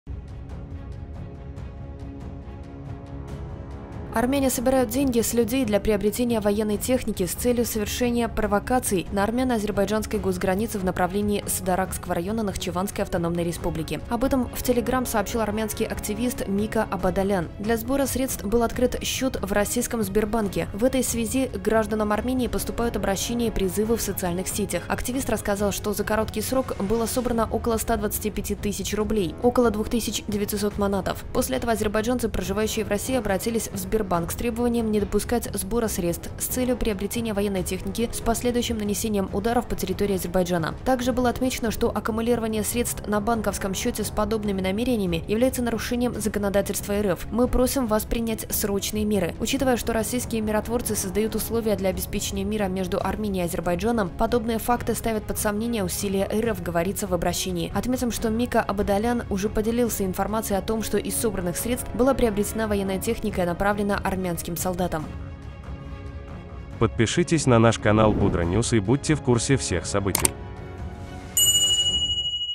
Армяне собирают деньги в российском банке с целью финансирования провокаций
📰Армяне собирают деньги с людей для приобретения военной техники с целью совершения провокаций на армяно-азербайджанской госгранице в направлении Садаракского района Нахчыванской Автономной Республики.

Об этом в Telegram сообщил армянский активист Мика Абадалян.

Для сбора средств был открыт счет в российском "Сбербанке". Активист рассказал, что за короткий срок было собрано около 125 000 рублей (около 2 900 AZN).

После этого азербайджанцы, проживающие в России, обратились в "Сбербанк" с требованием не допускать сбора средств с целью приобретения военной техники с последующим нанесением ударов по территории Азербайджана. Также было отмечено, что аккумулирование средств на банковском счете с подобными намерениями является нарушением законодательства РФ.

"Мы просим вас принять срочные меры. Учитывая, что российские миротворцы создают условия для обеспечения мира между Арменией и Азербайджаном, подобные факты ставят под сомнение усилия РФ", - говорится в обращении.

Отметим, что Мика Абадалян уже поделился информацией о том, что из собранных средств была приобретена военная техника и направлена армянским солдатам.